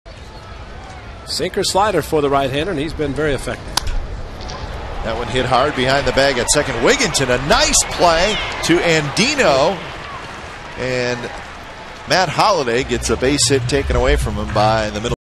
2009/06/07 Wigginton's hustling toss
BAL@OAK: Wigginton makes a nice running toss